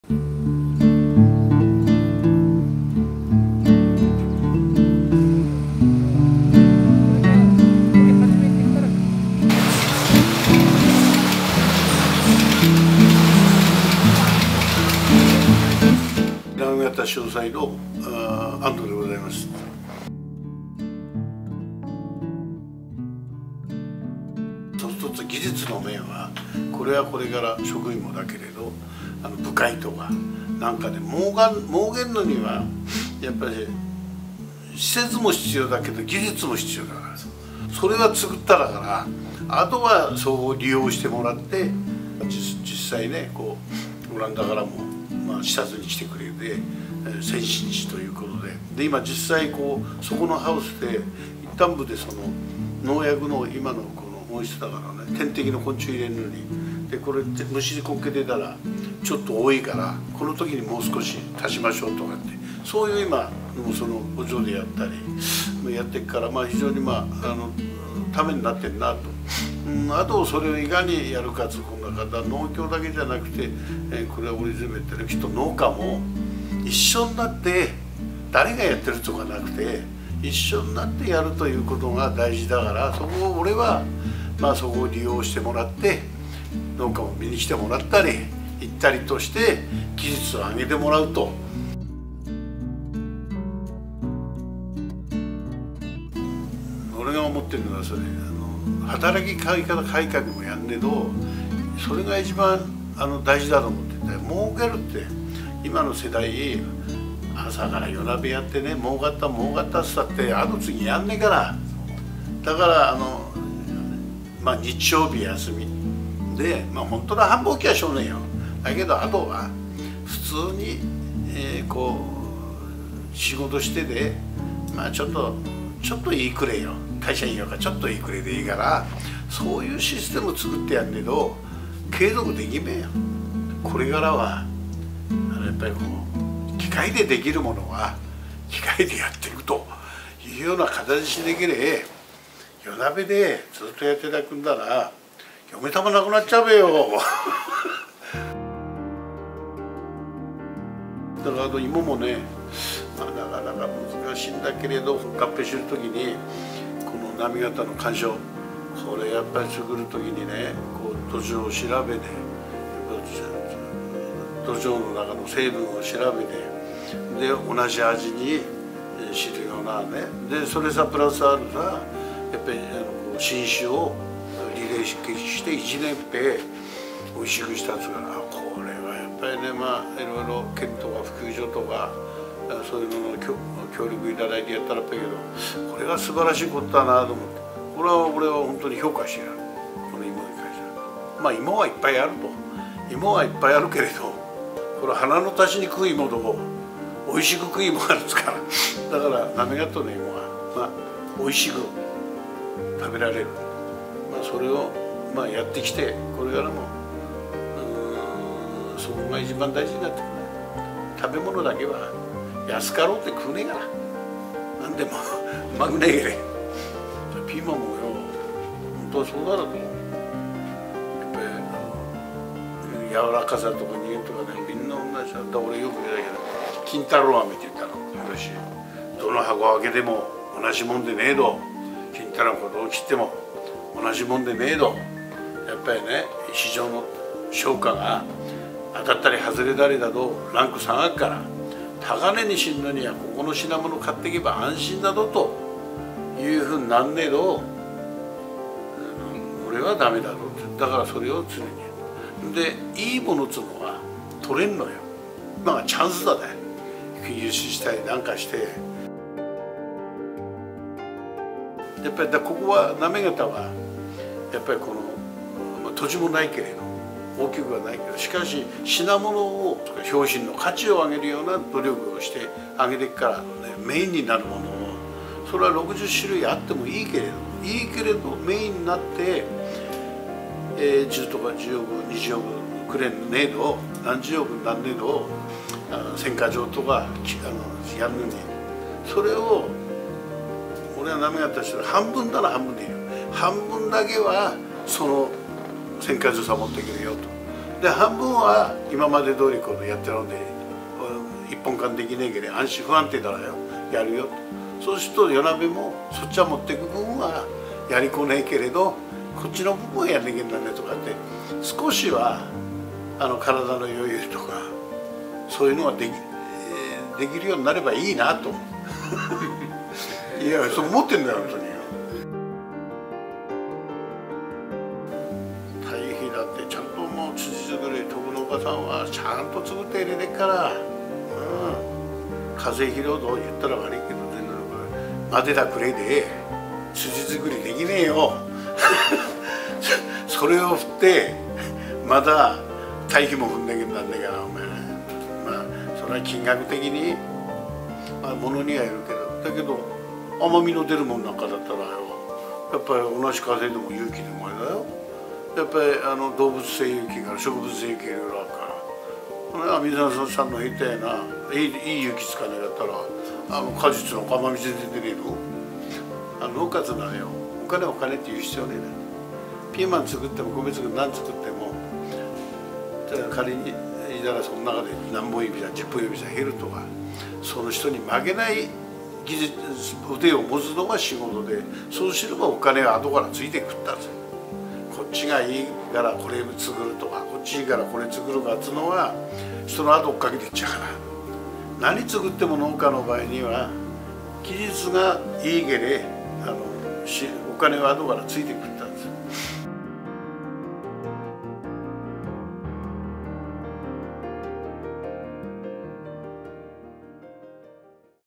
JAなめがたしおさい安藤組合長
２００億円を超える規模のJAなめがたしおさい。

安藤組合長に重点的に取り組んでいること、農業に関する思いなどを熱く語っていただきました。